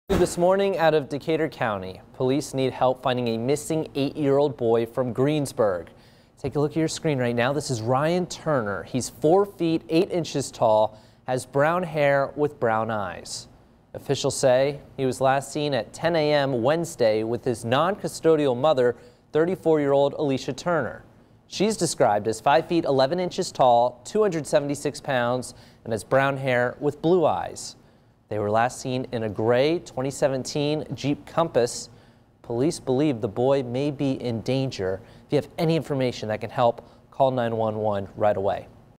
Silver Alert: Ryan Turner
Police need help finding a missing 8-year-old boy from Greensburg.